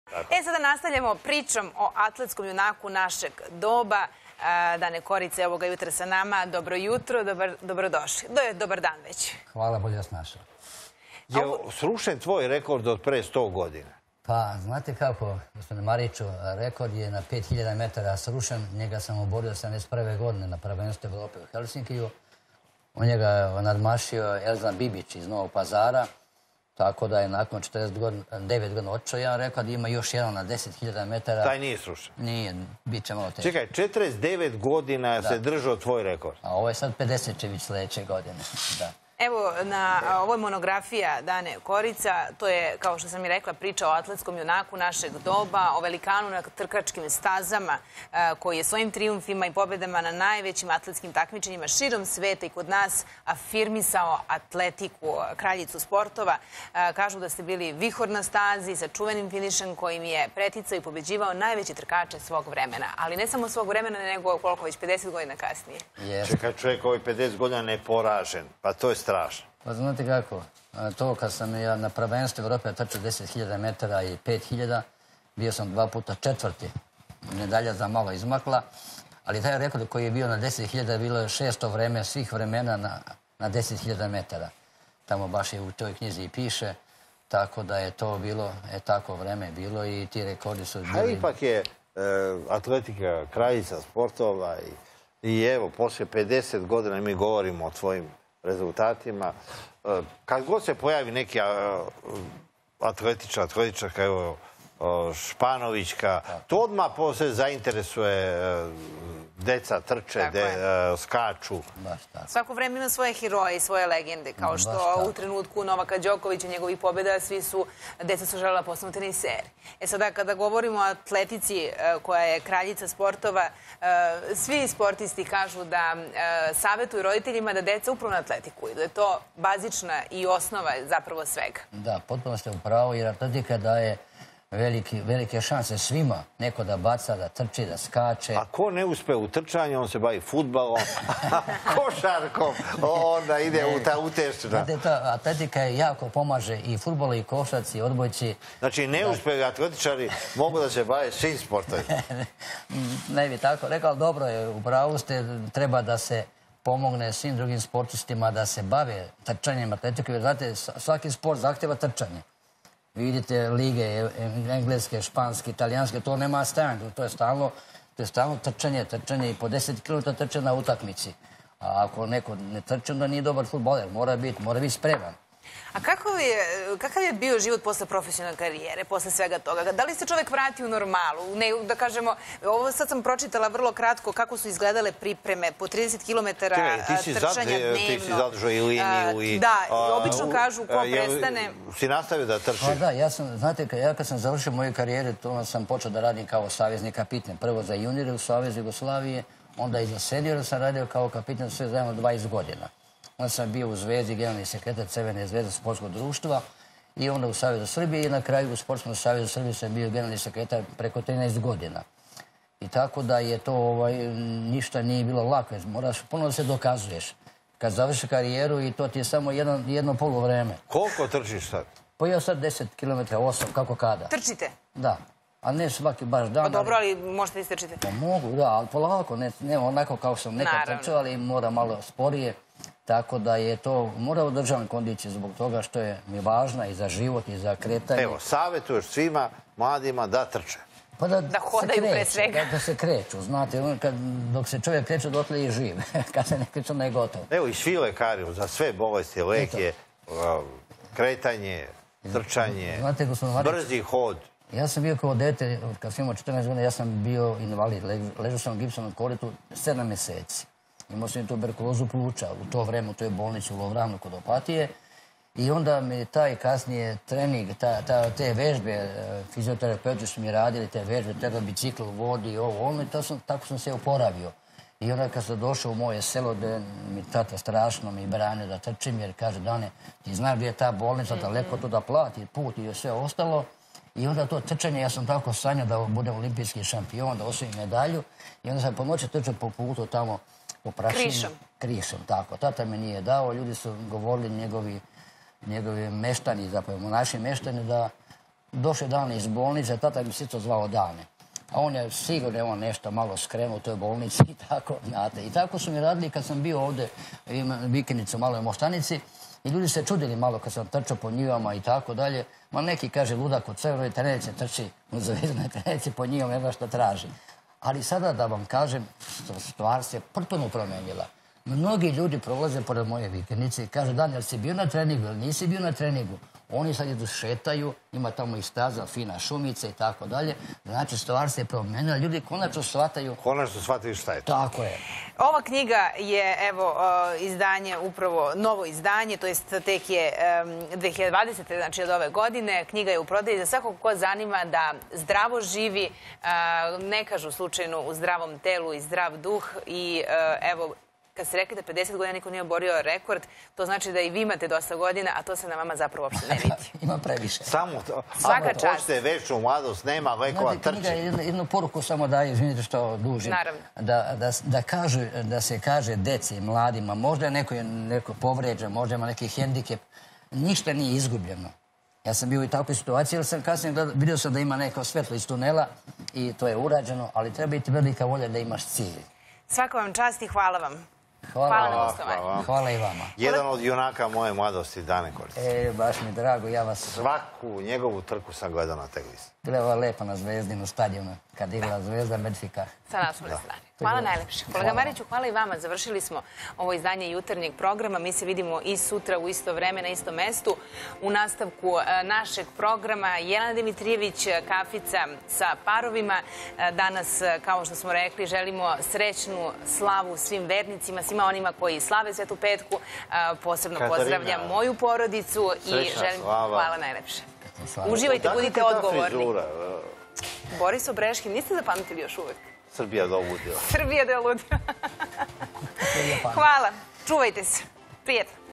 Dane Korica - Prica o atletskom junaku naseg doba koji je postavio velike rekorde - DJS - 27.10.2020
Gost: Dane Korica - Atletičar